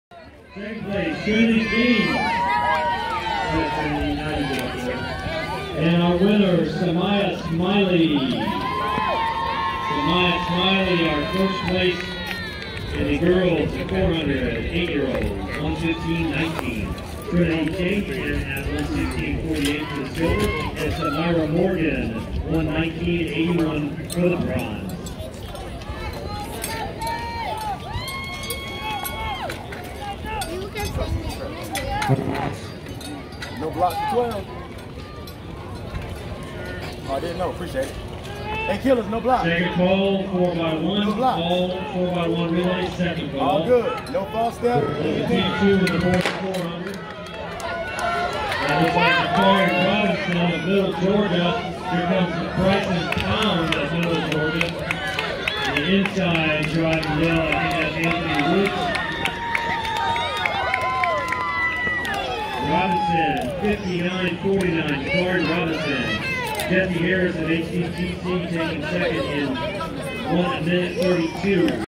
North Florida RoadRunners Boys 400m 14 Year Old (Heat 2)
V12 Invitational
Location: UNF Turf Field, Jacksonville, FL